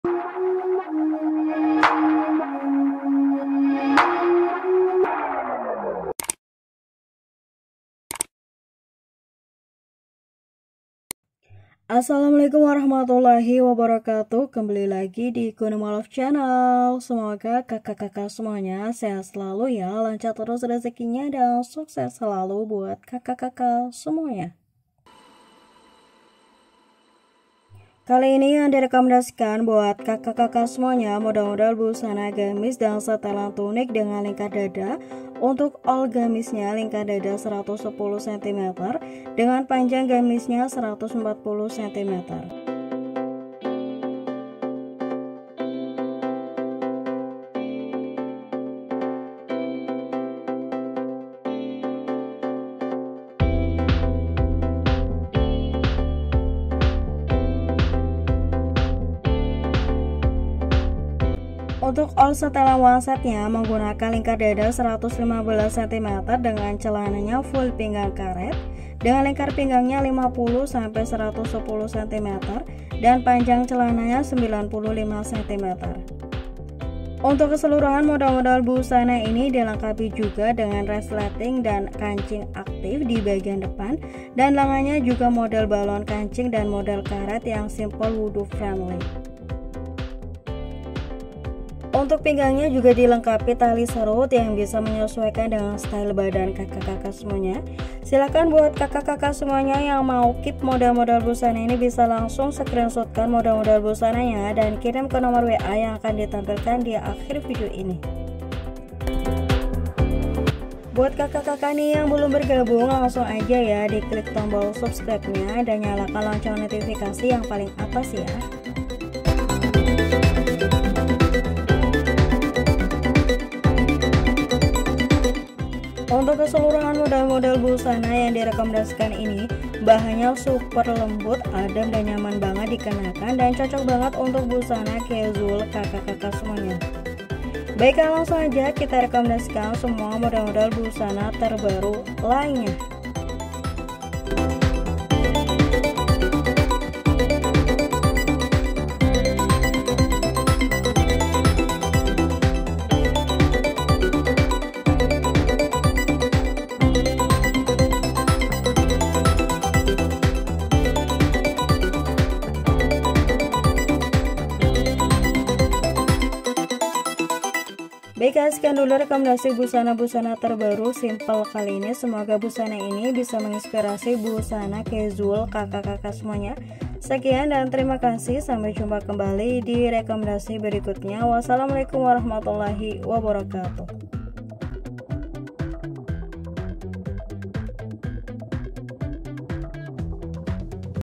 GAMIS SIMPLE KEKINIAN LD 110 COCOK UNTUK CASUAL
Assalamualaikum wr.wb
Kali ini yang direkomendasikan model model gamis simple kekinian terbaru yang cocok buat busana casual kakak kakak semua.Bahannya super adem ,lembut dan nyaman banget dikenakan untuk daily set,ataupun gamis casual .Ld 110-115 pj gamis 140 .Semoga busana simple ini menginspirasi kakak kakak semua.